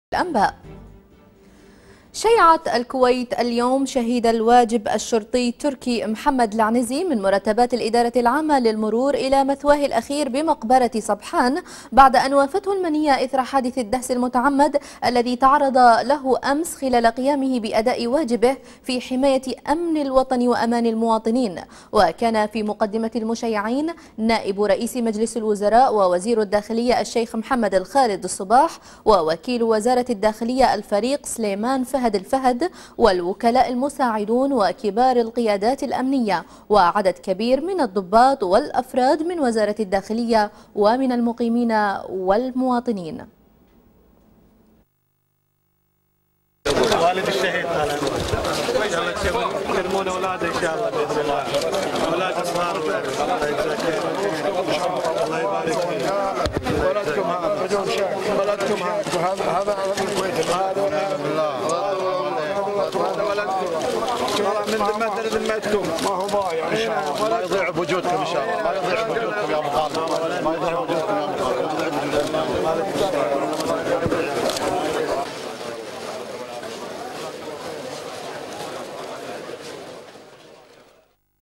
تلفزيون الكويت: الكويت تشيع شهيد الواجب الشرطي تركي محمد العنزي الى مثواة الاخير
الكويت,تشيع,شهيد,الواجب,الشرطي,تركي,العنزي,الى مثواة الاخير,وزارة,الاعلام,تلفزيون,الكويت